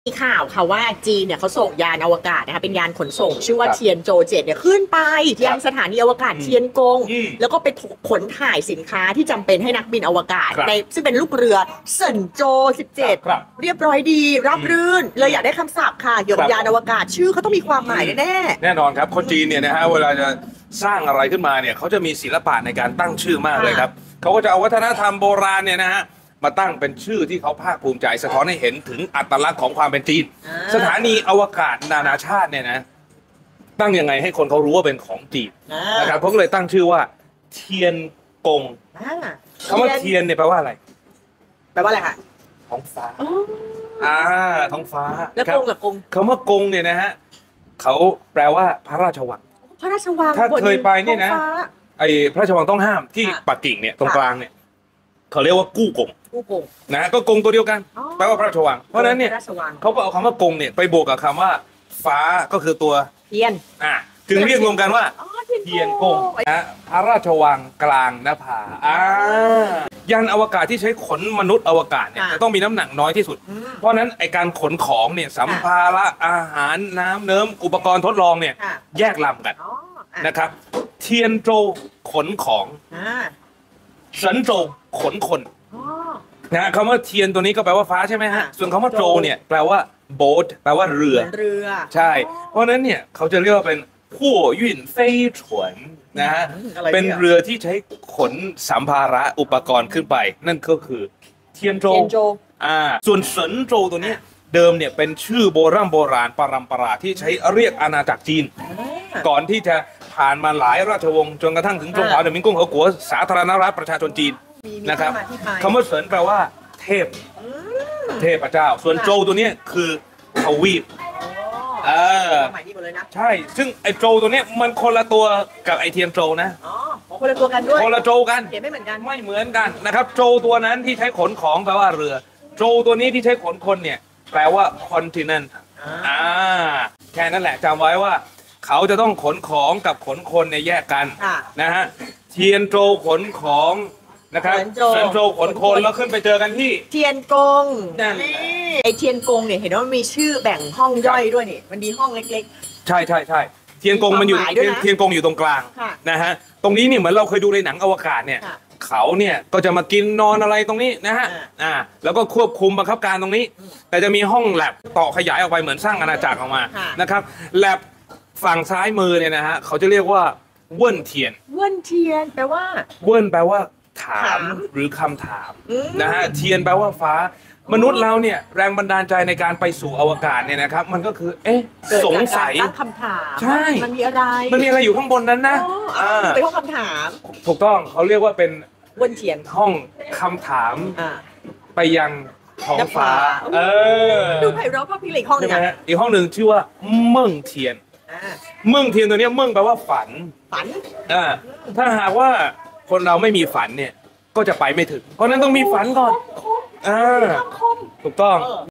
ศัพท์ ภาษาจีน เกี่ยวกับยานอวกาศของจีน
วันนี้มีศัพท์ ภาษาจีน ที่น่าสนใจ เกี่ยวกับยานอวกาศของจีนมาฝากกันค่ะ 😀 ชื่อ เทียนกง เทียนจิน เสินโจว มีความหมายว่าอย่างไร? ใครรู้บ้าง?